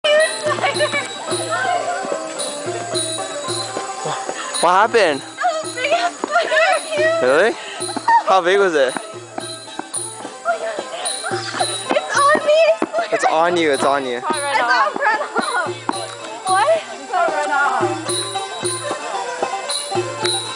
Ginger and her Phobia of Bugs
Ta Phrom Temple in Cambodia